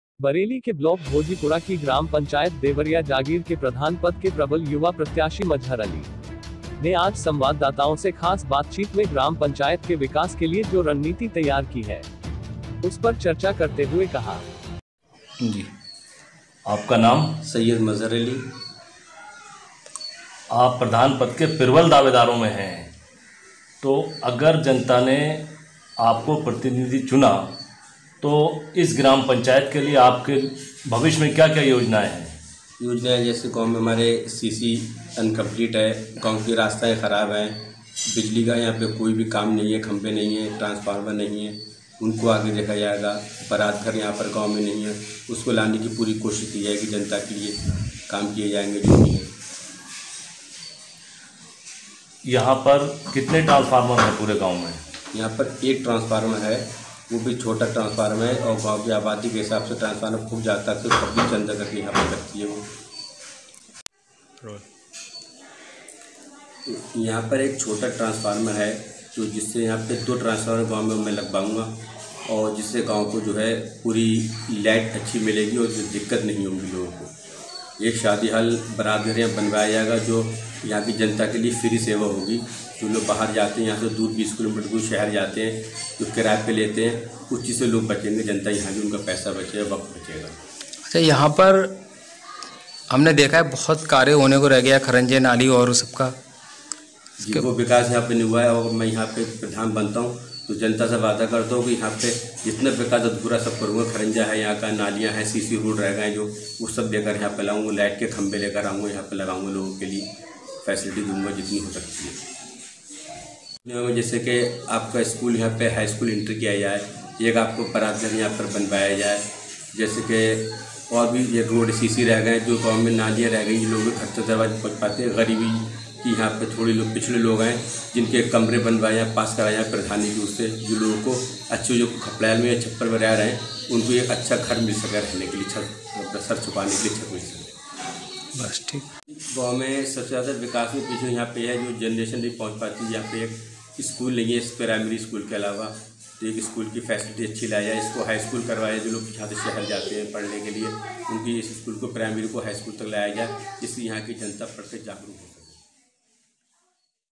ग्राम पंचायत देवरिया जागीर के प्रधान पद के प्रबल युवा प्रत्याशी मजहर अली से खास बातचीत
KHOJI CAMERA

बरेली के ब्लॉक भोजीपुरा कि ग्राम पंचायत देवरिया जागीर के प्रधान पद के प्रबल युवा प्रत्याशी मजहर अली ।

ने आज संवाददाताओं से खास बातचीत में ग्राम पंचायत के विकास के लिए जो रणनीति तैयार की है ।

उस पर चर्चा करते हुए कहा